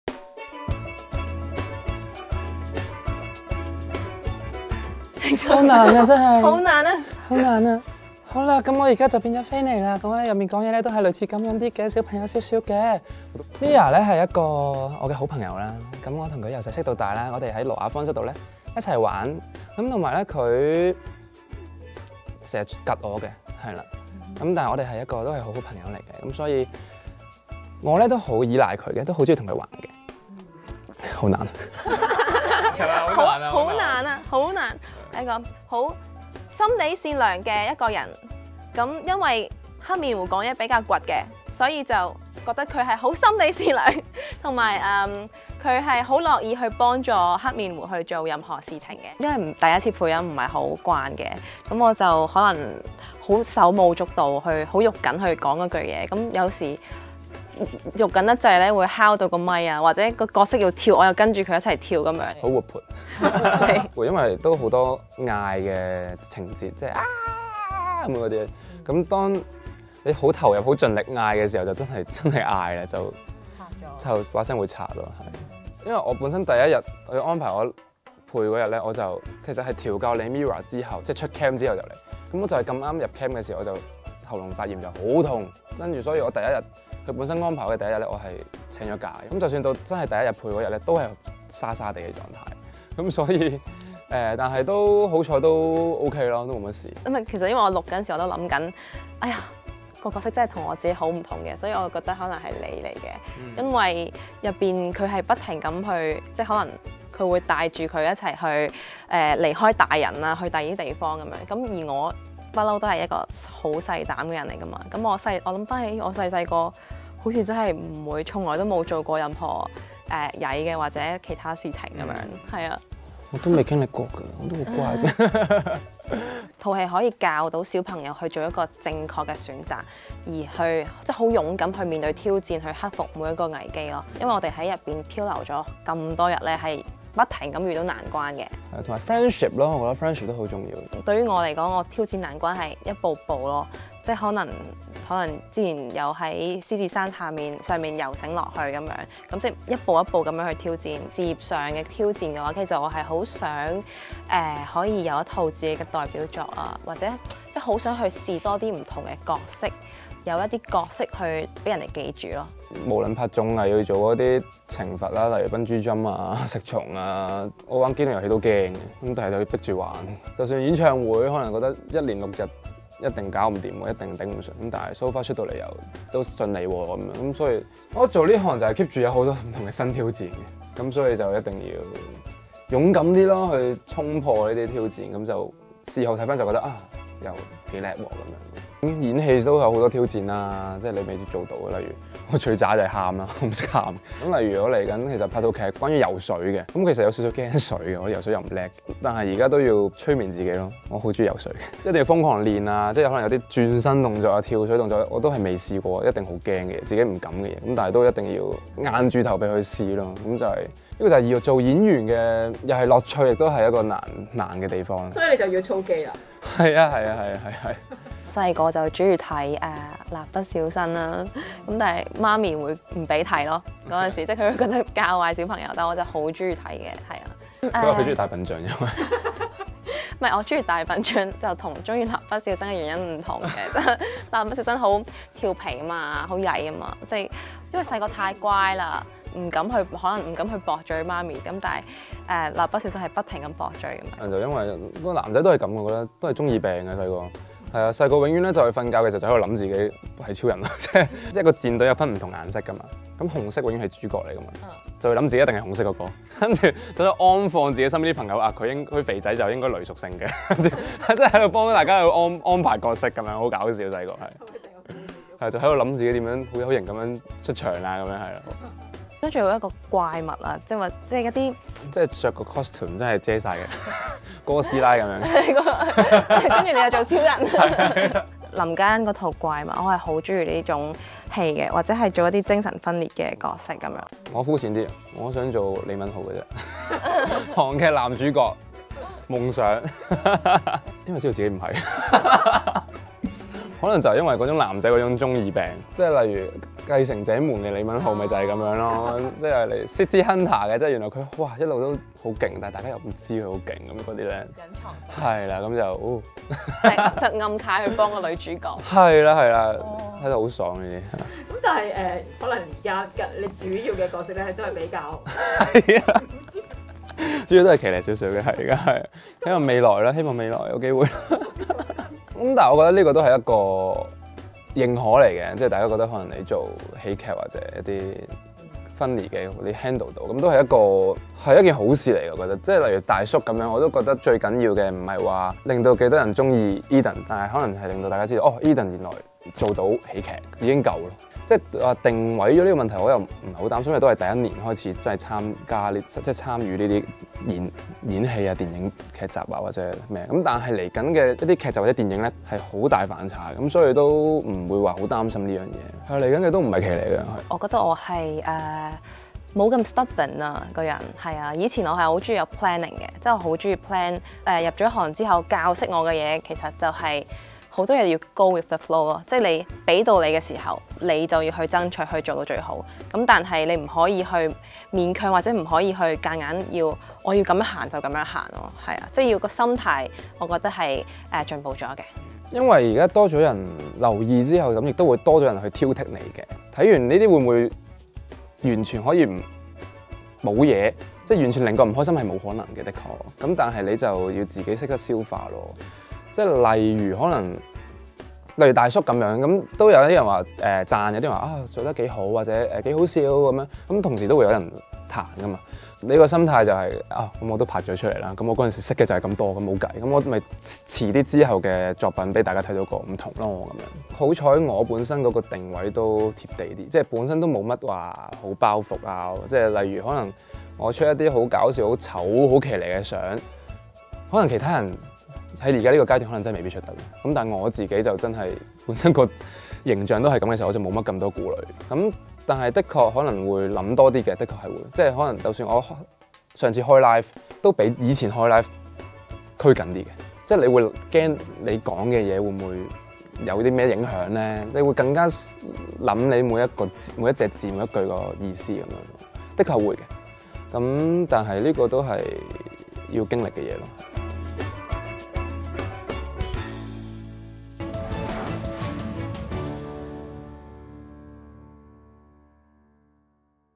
【娛樂專訪】孖陳欣妍首為動畫配音化身「童聲安」 Edan自認中二病
MIRROR成員呂爵安(Edan)在幕前的綜藝喜感，從《MIRROR GO》萌芽到《大叔的愛》的「田田」表情包大放送，盡顯娛樂大家的本領。最近，Edan與吳君如、鄭中基等拍賀歲片《闔家辣》進軍影壇後，更與陳欣妍(Shirley)雙雙為動畫《反轉方舟動物團》首度獻聲，兩人角色上身做聲演示範，竟頻頻高呼好難好難……其後更演變成「哥斯拉大戰超人」。

下周一(23日)Edan將會投入拍攝ViuTV劇集《i.Swim》，他接受本報專訪時表示，正為全新的游水劇操肌練水，不過要變身「大隻安」必先要克服恐懼，他自爆，「我有點怕水又不擅長游水，惟有催眠自己好鍾意游水，要瘋狂練習轉身或跳水動作。」一直落力做「小諧星」的Edan更透露下半年將會「轉型」暫別騎呢。至於演戲的終極目標？他笑言要向韓男Oppa李敏鎬進發！

bmgproductionmusic